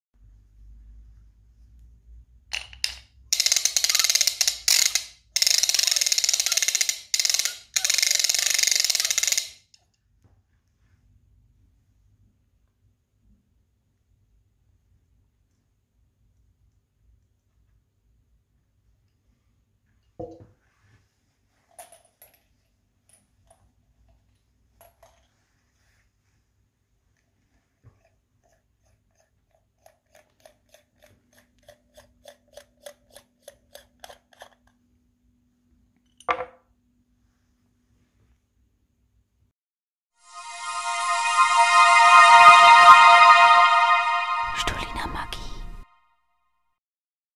Comedy Ring Polierer
Für Ringe, Münzen u. a. Dinge, die 'poliert' werden müssen! Der Ring des Zuschauers kommt in das Poliergerät und nach einigen lautknackenden Umdrehungen wird der Ring in die Hand des Zuschauers gegeben. Jetzt allerdings in Form von Gold- oder Silberstaub. Den Ring können Sie an beliebiger Stelle erscheinen lassen.